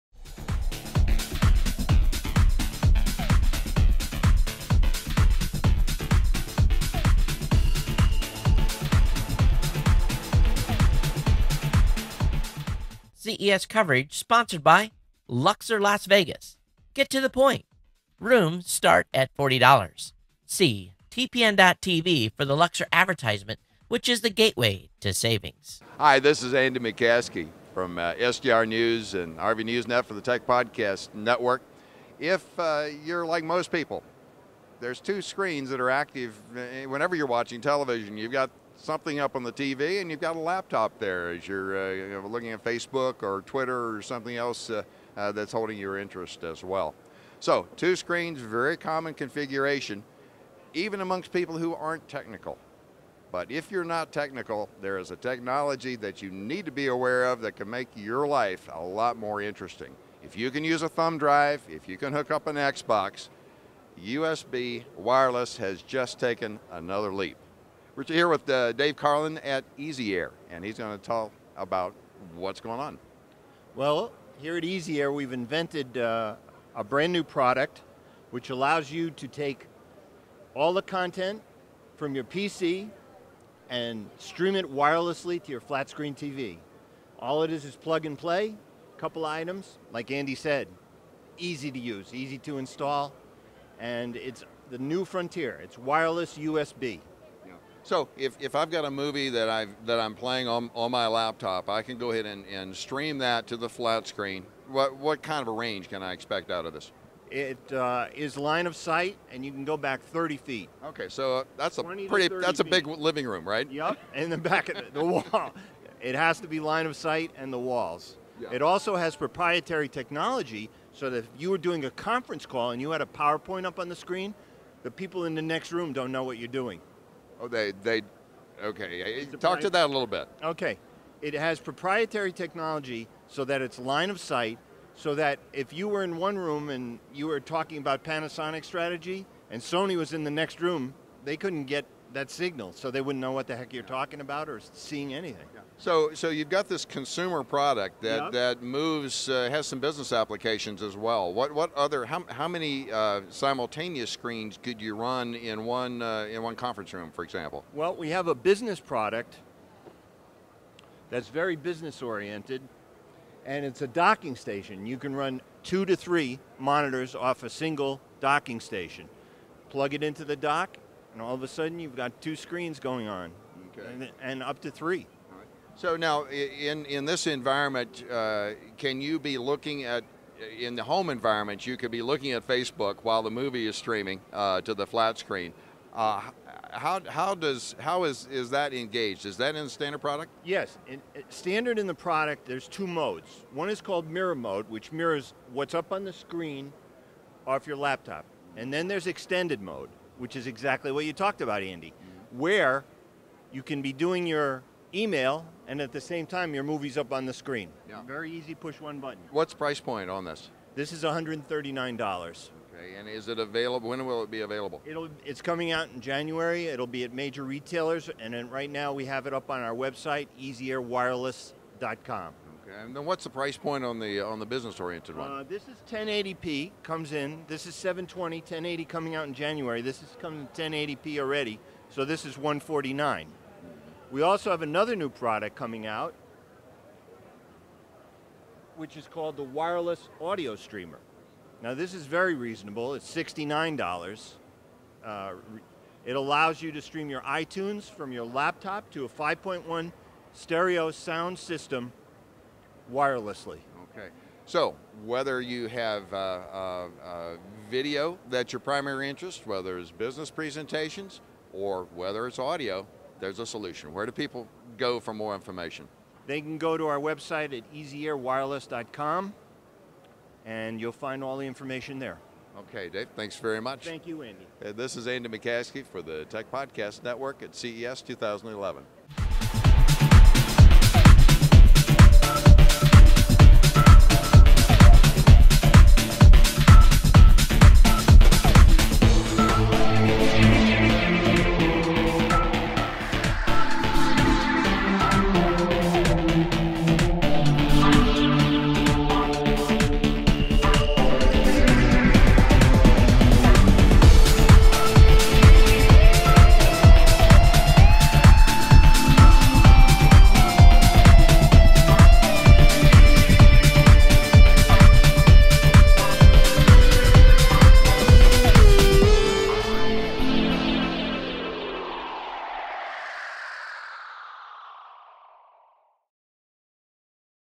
CES 2011 - EZAir
Wireless HDMI replacement adapts military wireless technology to home and business applications. The coverage is restricted to the room, and kept private from those in adjacent rooms.